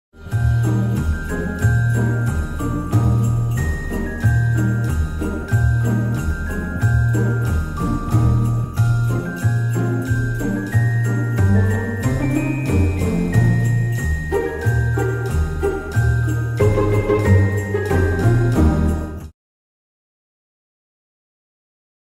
Photo's with Santa at RollerJam
Photo's with Santa at RollerJam, Saturdays in December. More fun than the mall at much lower price's.